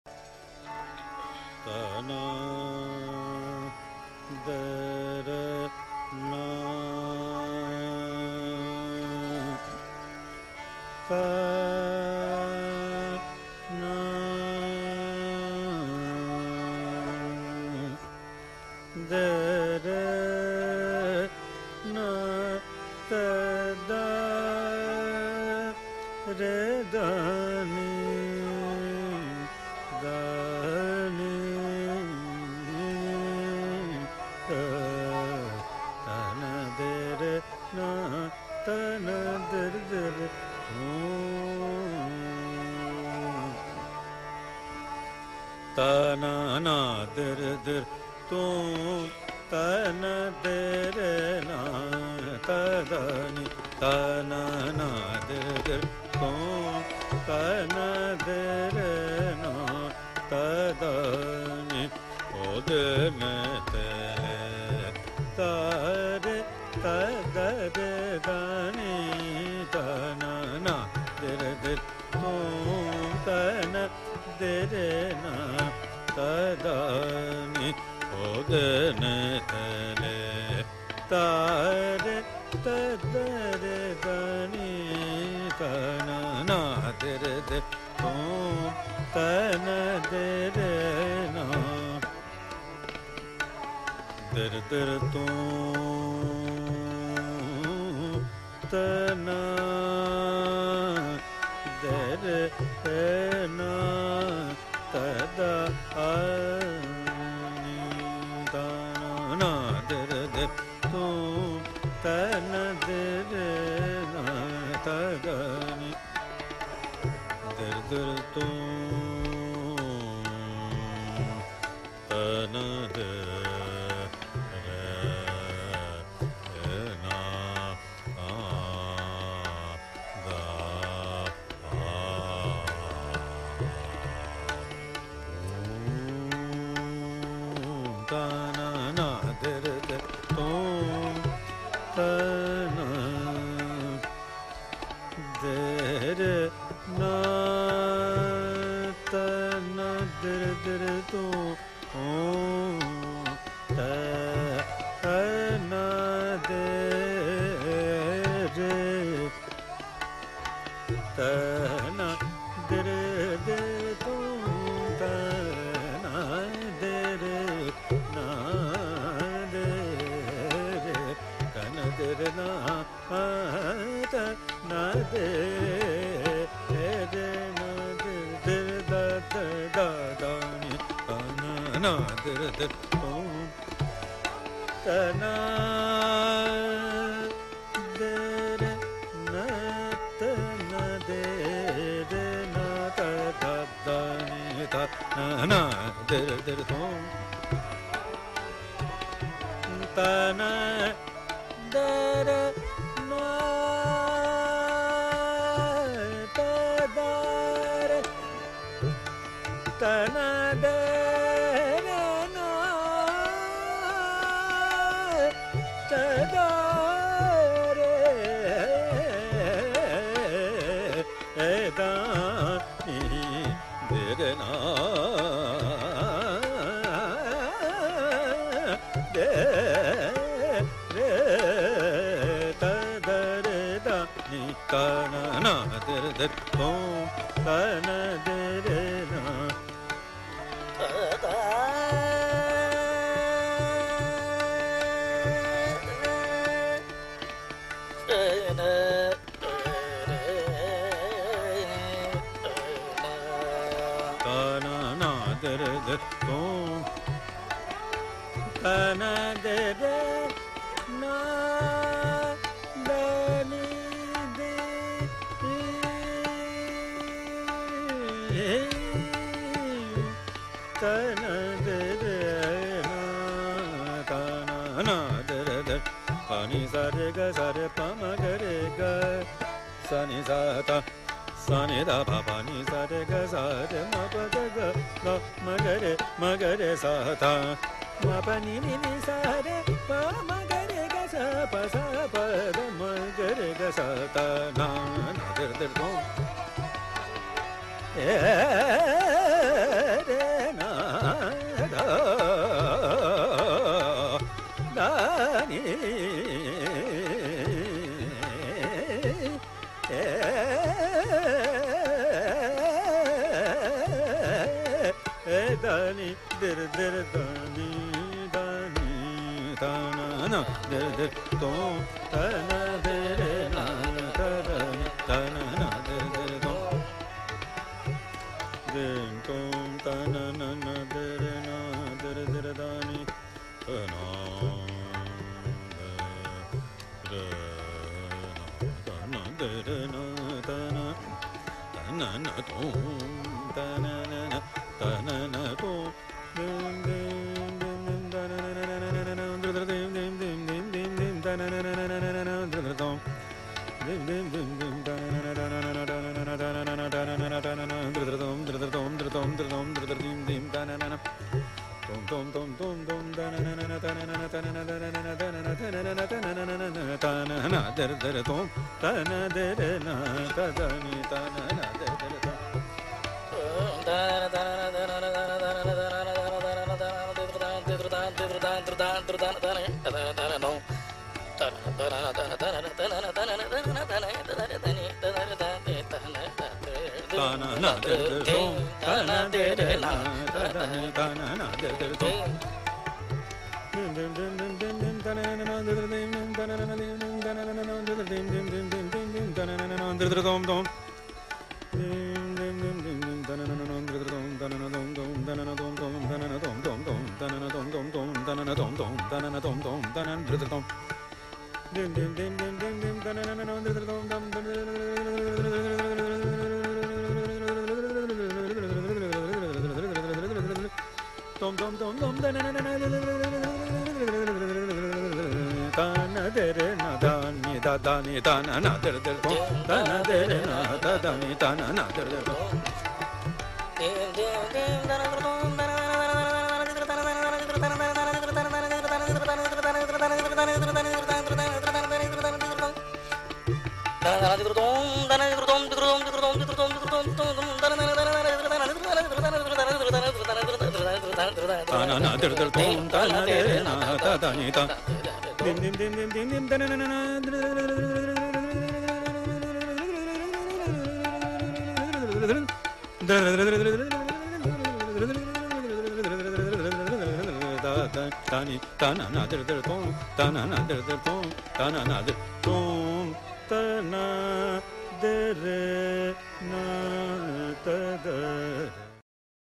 Ustad Hafeez Ahmed Khan - Tarana Raag Tilak kamod.avi
Ustad Hafeez Ahmed Khan singing Tarana